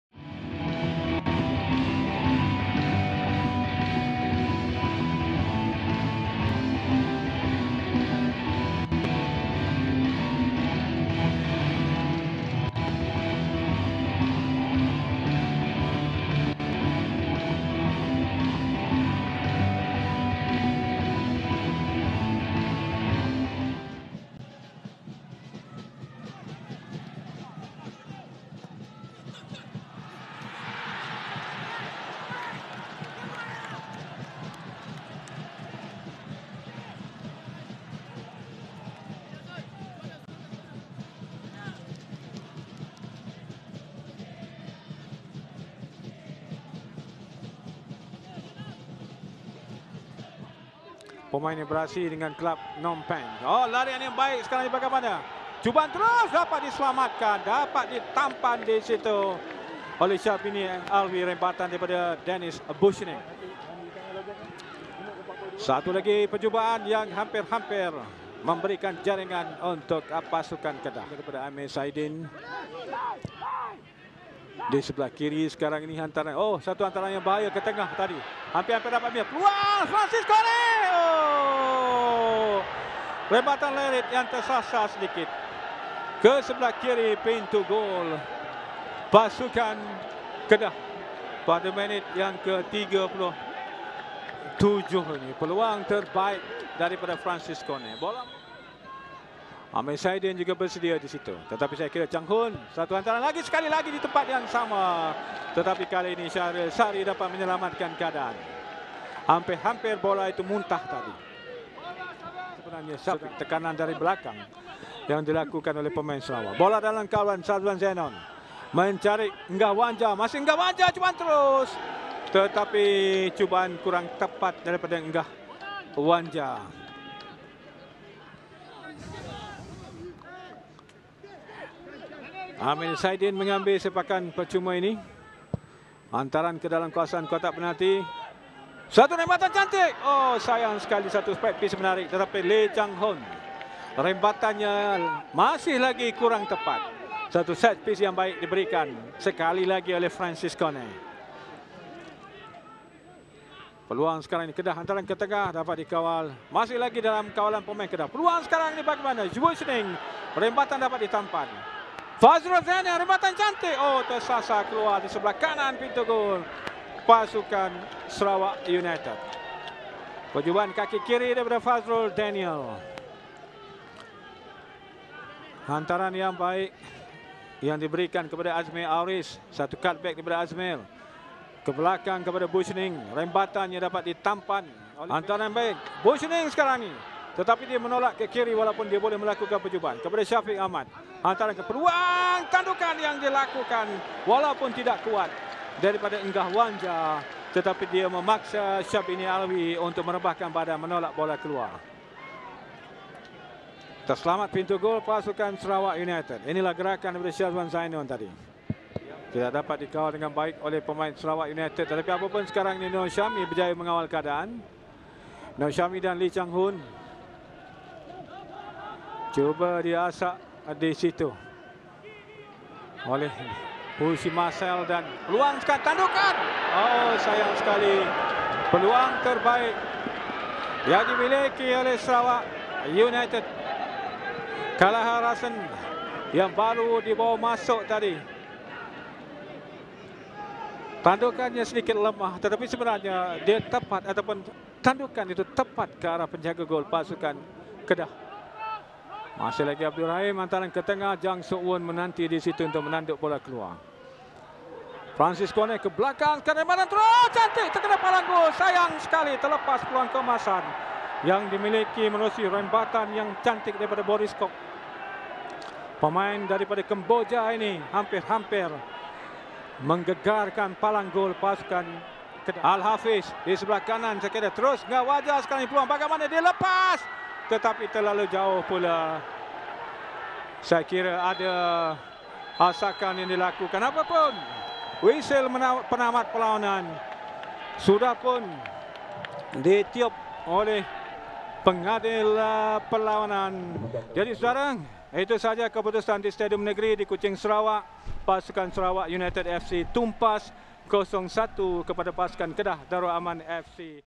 Sorotan Aksi Liga Super 2022 | SARAWAK UNITED 0-1 KEDAH FC
📹 HIGHLIGHTS: SARAWAK UNITED 0-1 KEDAH FC

Jaringan Ronald Ngah memastikan skuad Sang Kenari meraih kemenangan di Kuching.